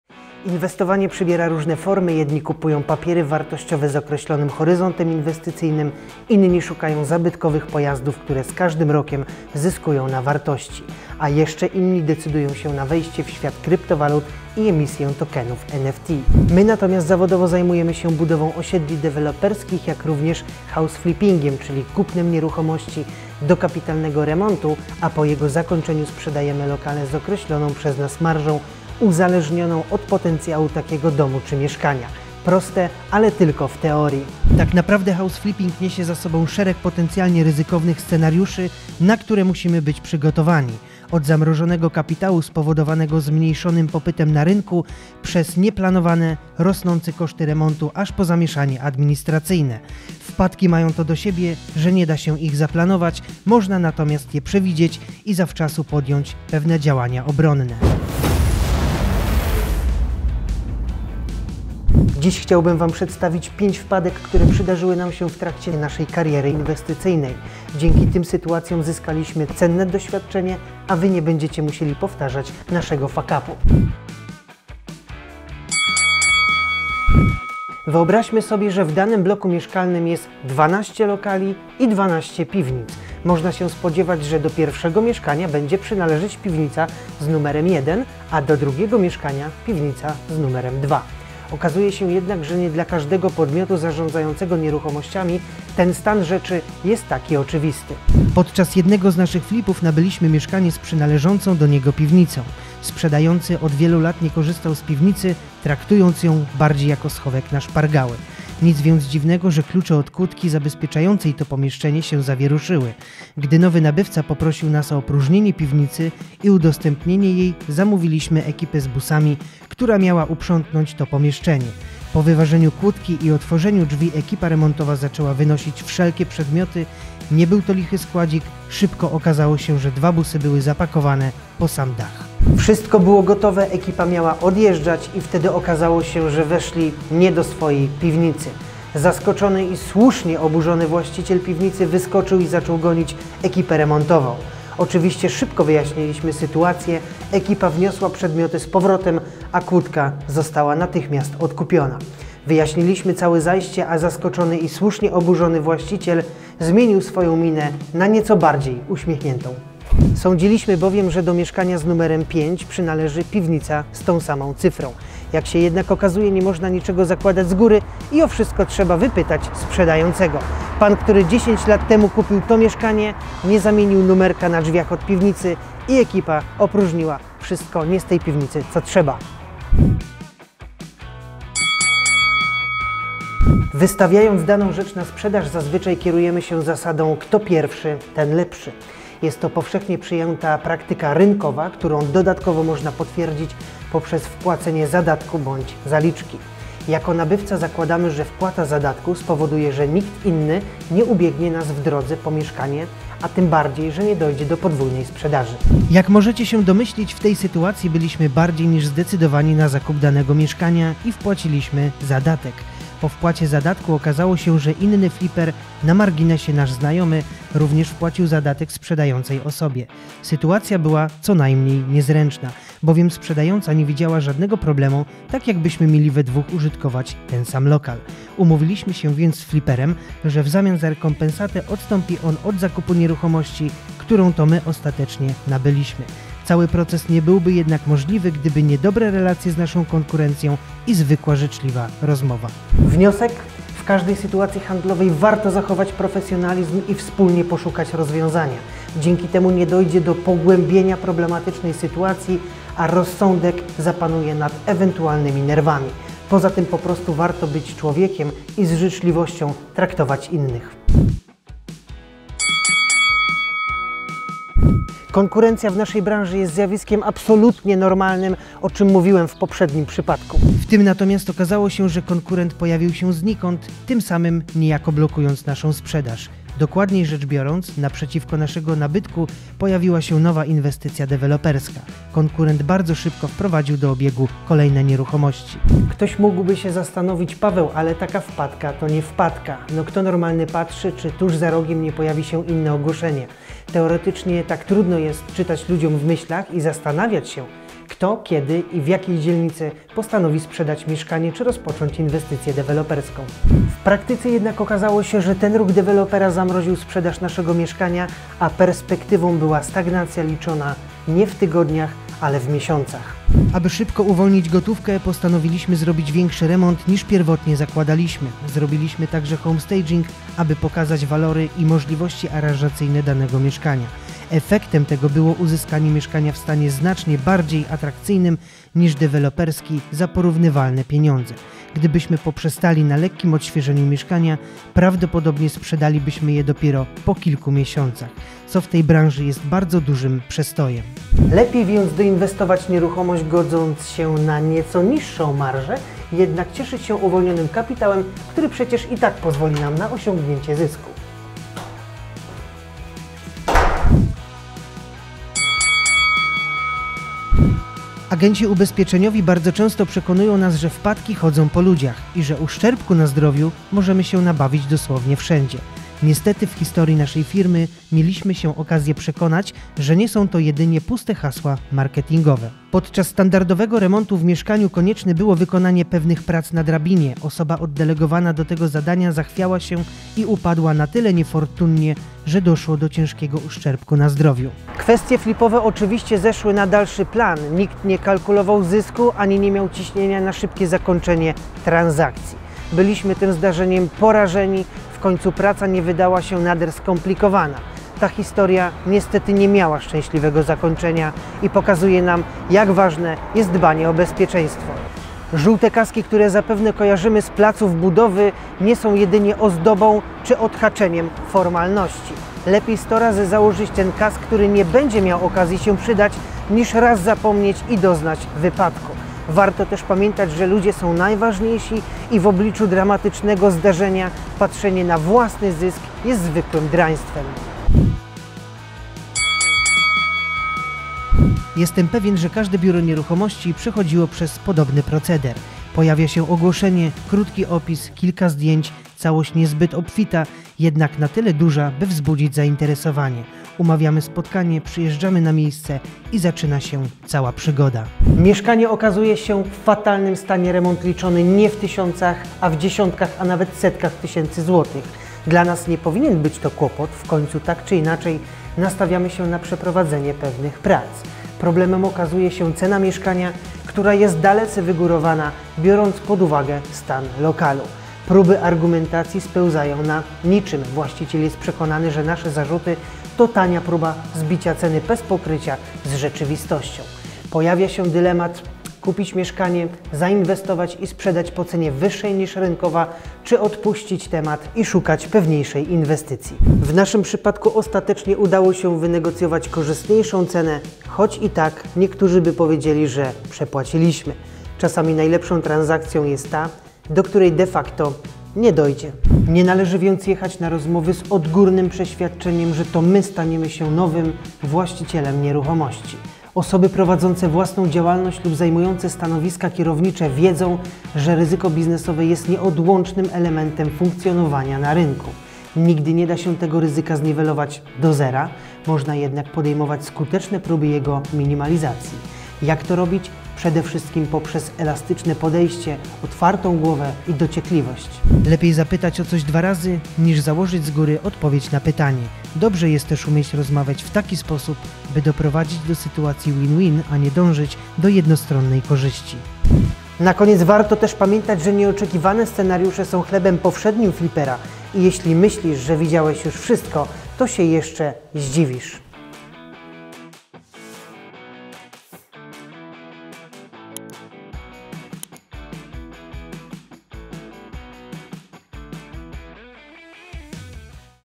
WPADKI FLIPPERSKIE - Jak robić flipy by uniknąć komplikacji?
Wiesz już na czym polega house flipping i myślisz, że to najprostsza forma inwestowania? Okazuje się, że niesie on za sobą szereg potencjalnie ryzykownych scenariuszy, na które musimy być przygotowani. Zmniejszony popyt i zamrożony kapitał, nieoczekiwane komplikacje przy remoncie mieszkania, problemy administracyjne z zarządcą - to tylko niektóre przykłady. Takie czynniki mają to do siebie, że nie da się ich zaplanować, ale można się do nich przygotować poprzez naukę na błędach, w tym błędach konkurencji, a także przez przygotowanie scenariuszy awaryjnych. Pamiętajcie - każda wpadka, to cenna nauka i wnioski na przyszłość. Zobaczcie 5 sytuacji, które przytrafiły się nam w ostatnich latach.

Odwiedź nas na:
A co znajdziesz w środku?
00:00 Start
00:15 Czym zajmuje się Horyzont Inwestycji?
01:14 Wkradli się do piwnicy Sąsiada!
03:00 Podwójna sprzedaż tego samego mieszkania.
04:21 Nieoczekiwana konkurencja.
05:53 Dramatyczny wypadek.
07:07 Pozorna okazja na flipa.